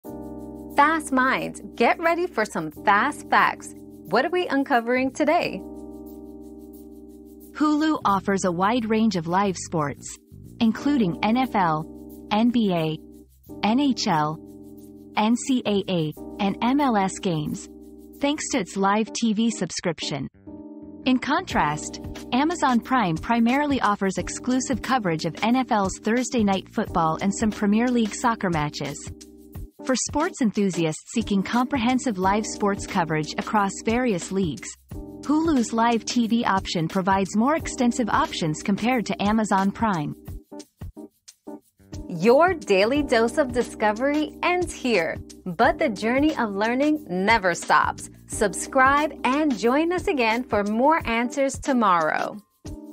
What types of live sports does Hulu offer compared to Amazon Prime?
Hulu vs. Amazon Prime: Which One Has More Live Sports? 👉 Live Sports Showdown 👉 Discover the range of live sports offered by Hulu and Amazon Prime. While Hulu provides a diverse selection of NFL, NBA, NHL, NCAA, and MLS games, Amazon Prime focuses mainly on NFL's Thursday Night Football and Premier League soccer. Find out which platform offers the best live sports coverage for you!

Laura S. Harris (2024, July 3.) What types of live sports does Hulu offer compared to Amazon Prime?
    🌐 AskAbout.video